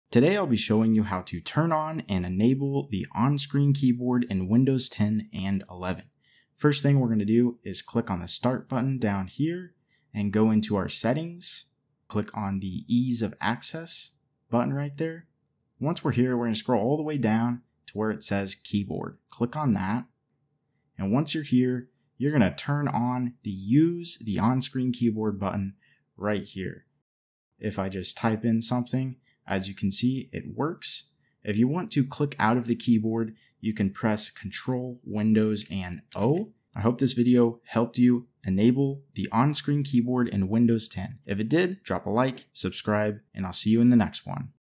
How to ENABLE On Screen Keyboard in Windows 10 | FIX Laptop Keyboard Not Working | Windows 10,11,8,7
How to enable and disable the on screen keyboard in windows 10 and 11.  This updated tutorial will help show you how to turn on and enable the on screen keyboard in windows 10, if your keyboard is not working on windows 10.  This tool is very useful and can help users if their keyboard is not working properly on their laptop and PC.  This allows users to input text using a virtual keyboard to type and allows users to fix laptop touchpad issues and if touchpad is not working when mouse is connected.  To start, click on the windows start button, then click on settings.  You will then click on ease of access.  From there you will click on keyboard, then turn on the on screen keyboard for windows 10 and 11.  You can also use the quick command to enable and disable the on screen keyboard: *Ctrl + Windows + O*

▶️ *For Potential Partnerships:*
👉If you're a company or brand interested in partnering with me, fill out our inquiry